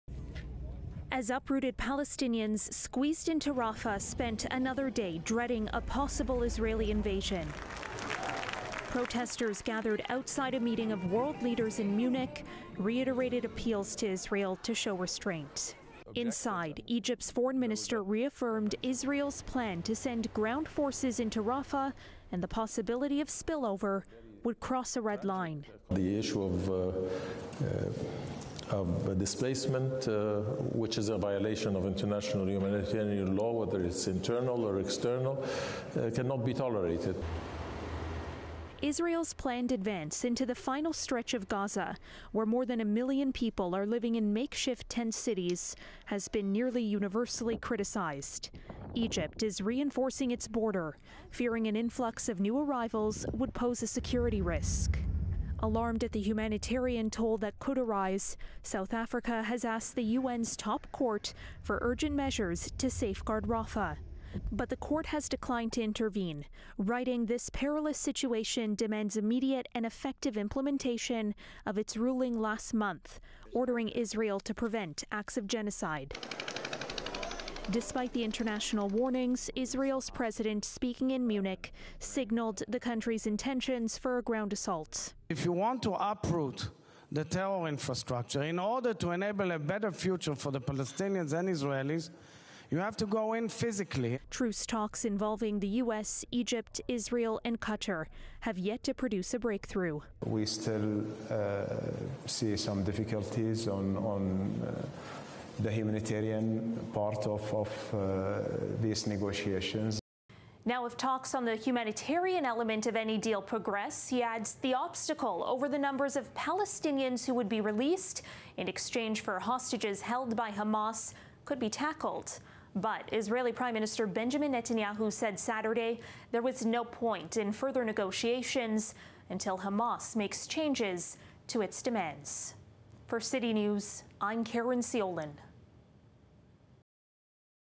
Rafah offensive looms large at Munich Security Conference
The U.N.’s top court has rejected a request to safeguard the southern Gaza city that Israeli forces are poised to invade. Caryn Ceolin with the appeals for restraint in Rafah as Israel reaffirms its intentions.